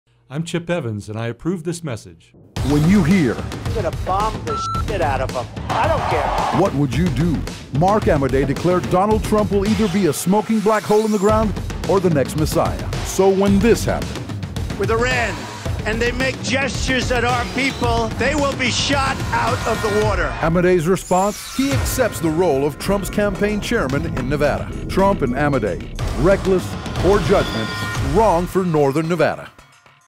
Chip Evans 4 Congress - "Judgment" TV Spot (English)
Trump + Amodei. Reckless. Poor Judgment. Wrong for Northern Nevada.